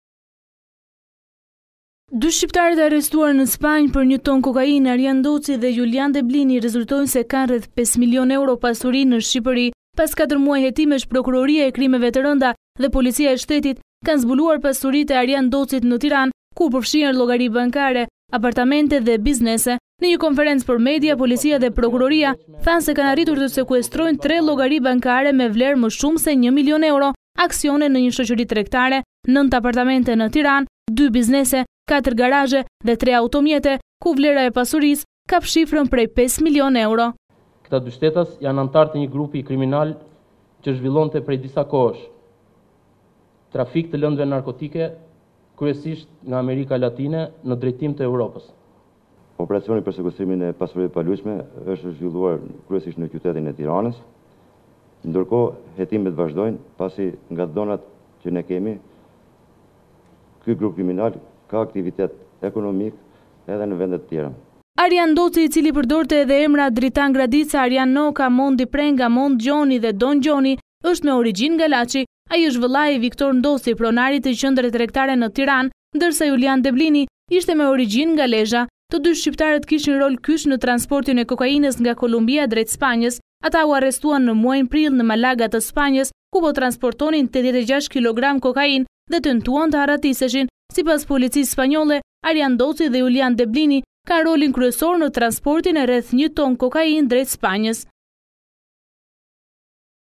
U arrestuan me 1 ton kokainë në Spanjë, sekuestrohen 5 mln euro pasuri e 2 bosëve nga Laçi dhe Lezha
Policia e Shtetit dhe Prokuroria e Krimeve të Rënda kanë sekuestruar 5 milionë euro pasuri e një grupi kriminal shqiptar, të cilët janë arrestuar në muajin prill 2018, me 1 ton kokainë në Spanjë. Eduart Merkaj, kreu i Policisë Kriminale, në një deklaratë për mediat ka thënë se Forca e Ligjit ka sekuestruar pasuritë e Arjan Ndocit dhe Julian Deblinit, nga Laçi dhe Lezha. Ata u arrestuan me veprën penale të trafikut ndërkombëtar të lëndëve narkotike, ndërsa Merkaj theksoi se janë anëtarë të një grupi që vepronte prej vitesh në trafikun e kokainës nga Amerika Latine në Evropë. Ditën e sotme, Task Forca ka sekuestruar disa llogari bankare, kuotat e një shoqërie tregtare, 1 njësie banimi, 8 apartamente, 2 njësi tregtare, 4 garazhe dhe 3 makina. Asetet kriminale kapin vlerën e 5 milionë eurove.Njoftimi i plotë:

Forca e Ligjit” në veprim sekuestrohen 5 mln euro asete të një grupi kriminal,
me aktivitet në fushën e trafikimit ndërkombëtar të lëndëve narkotike,
duke realizuar operacionin policor “Vazhdimi”

Më datë 30 prill 2018, autoritetet policore spanjolle në bashkëpunim DEA dhe EUROPOL kanë arrestuar në flagrancë shtetasit shqiptarë:

• Arjan Ndoci, alias (Dritan Gradica, Ardjan Noka, Mondi Prenga, Mond Gjoni, Don Gjoni,) 41 vjeç, lindur dhe banues në Laç Kurbin;

• Julian Deblini (Bedlini), vjec 35, lindur dhe banues në Lezhë.

Ata akuzohen për veprën penale të “Trafikimit ndërkombëtar të lëndëve narkotike”, pas kapjes së një sasie prej 1.000 kg kokainë, e cila dyshohet se vinte nga Kolumbia si dhe të një shume prej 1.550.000 Euro cash.

Këta 2 shtetas janë anëtarë të një grupi kriminal që zhvillonte prej kohësh trafik të lëndëve narkotike nga vendet e Amerikes Latine në drejtim te Europës.